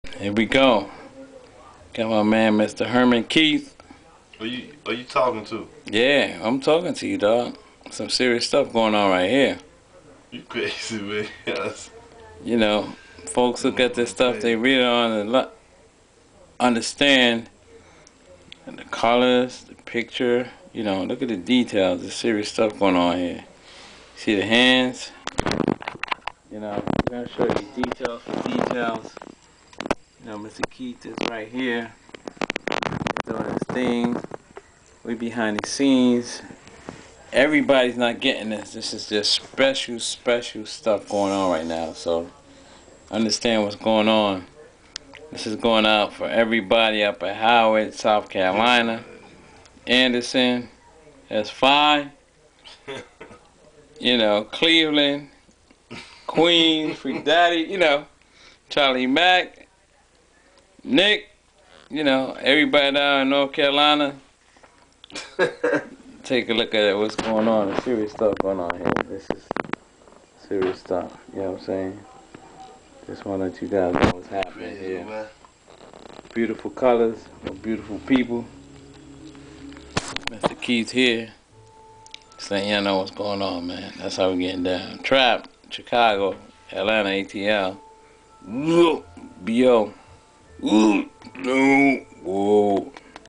Herman Keith Art Work
Artist Extraordinaire  Herman Keith displaying some of his art work while Donsolis is doing the video and voice over.